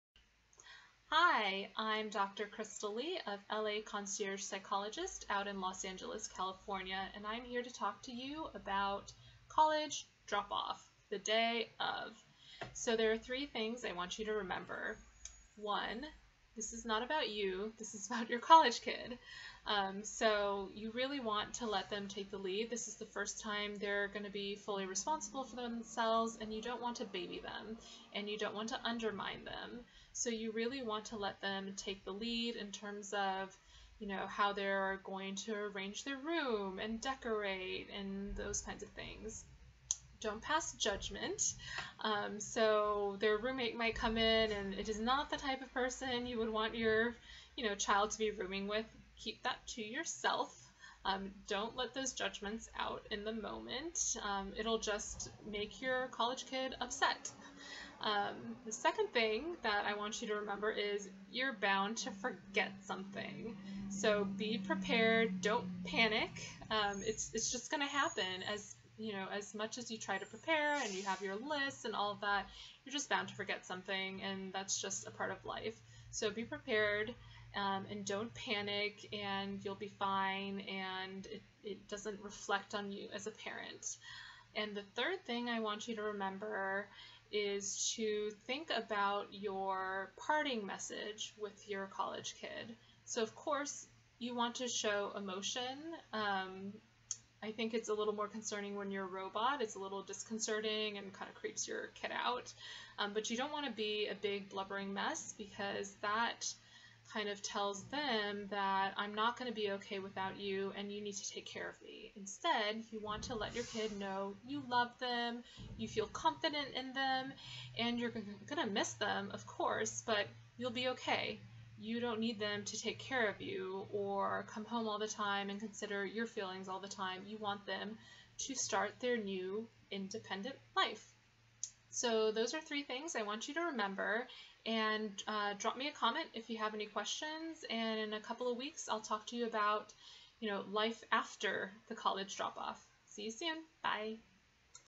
Saying Goodbye to Your College-Bound Kid (Part Two of Three): The Day Of College Drop Off
The day of college drop off is filled with a swirl of competing emotions-- you're proud of your emerging adult, and you're going to miss them like crazy. You're happy... and you're sad. You see your child becoming an awesome adult, and you wish you could hold on to him forever. This video will give you three main considerations for the day of college drop off.